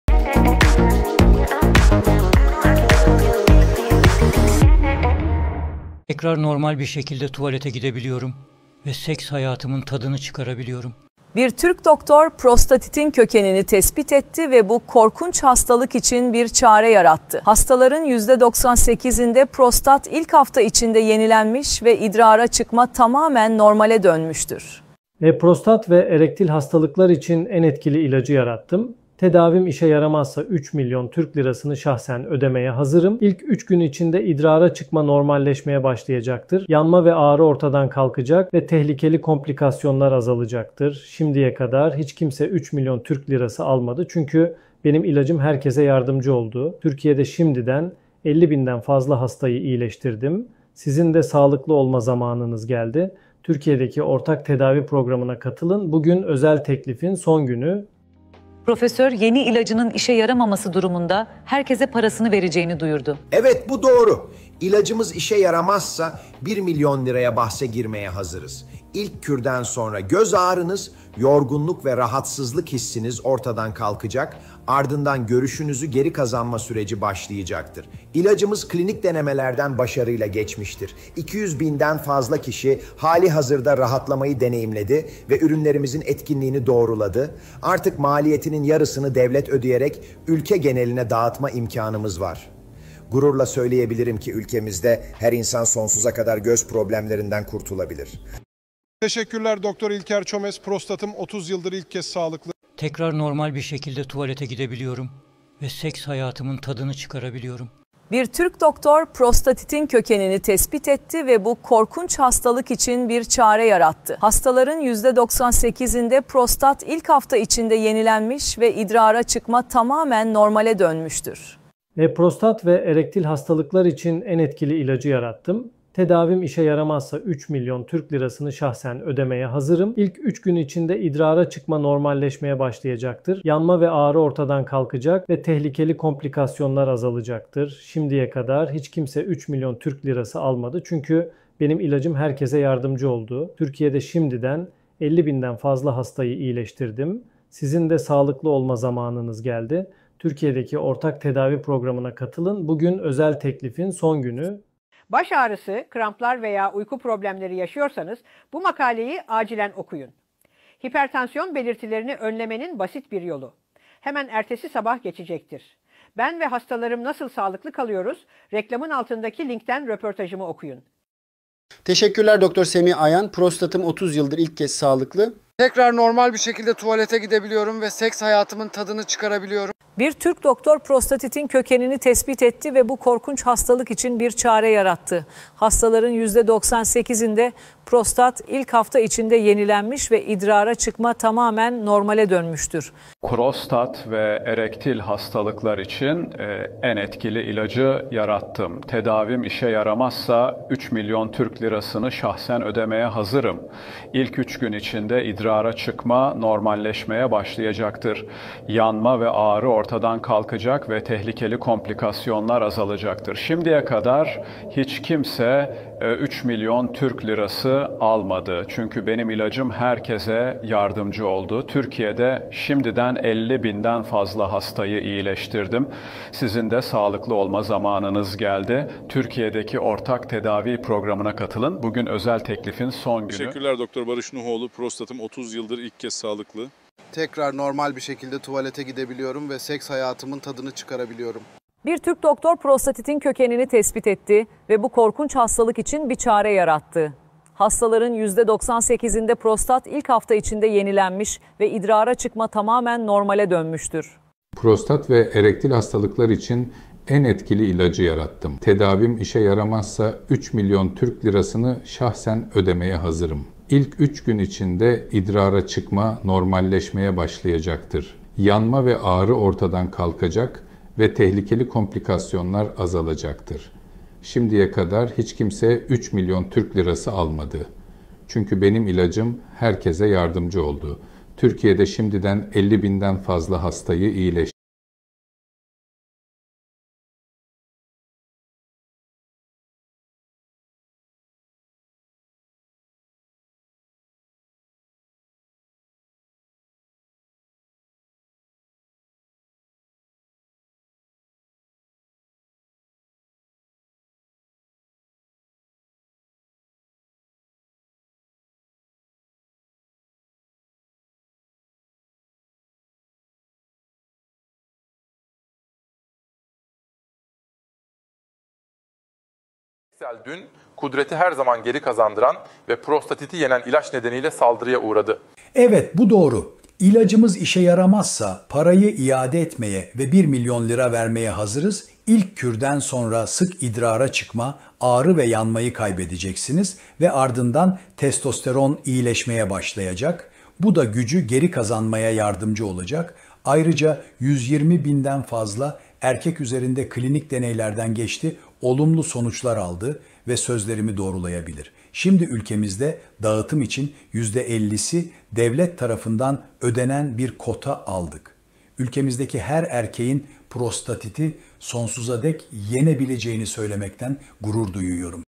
Yapay Zeka Dolandırıcılarını İfşa Ediyoruz(3): Gerçek Görüntüler ve Analizler!
Dolandırıcılar, ünlü kişilerin ve tanınmış isimlerin ses ve görüntülerini taklit eden yapay zekâ teknolojilerini kullanarak kurbanları kandırmaktadır.